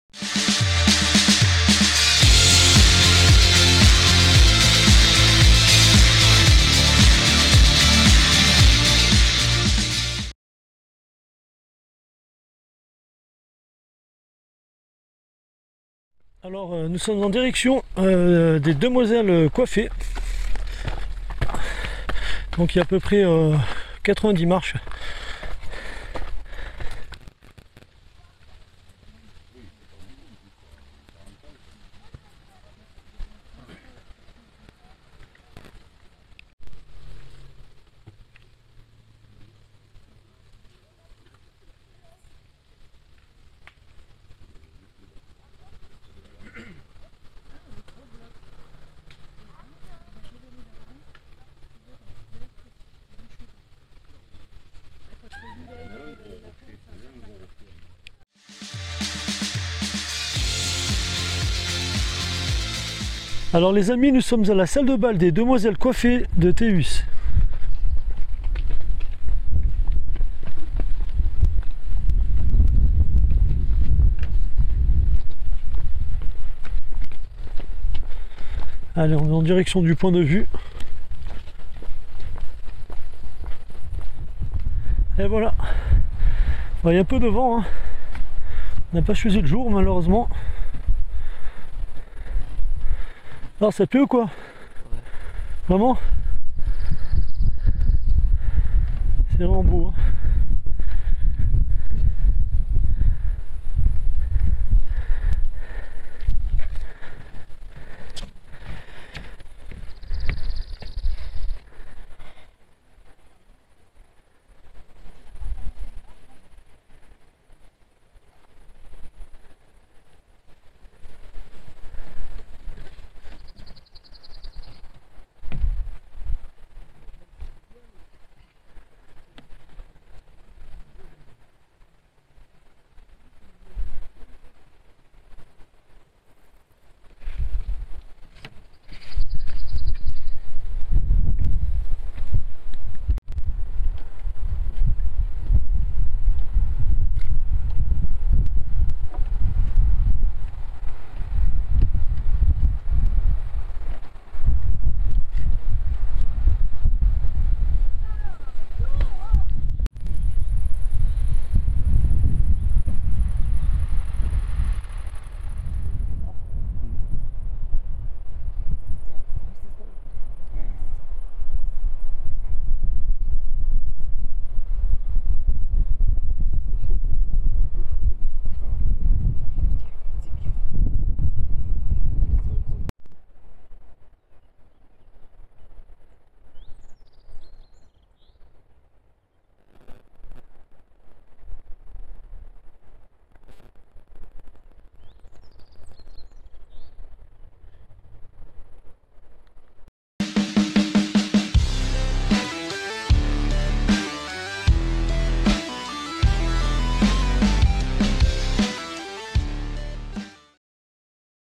[Patrimoine] Les demoiselles coiffées de Pontis et la salle de bal des demoiselles coiffées de Théus
Antony et Moi Visitons les Demoiselles coiffées de Pontis et La salle de bal des demoiselles coiffées de Théus Lors de notre Road Trip du Mois de Juillet 2021.
🔔✅Actives la cloche de notification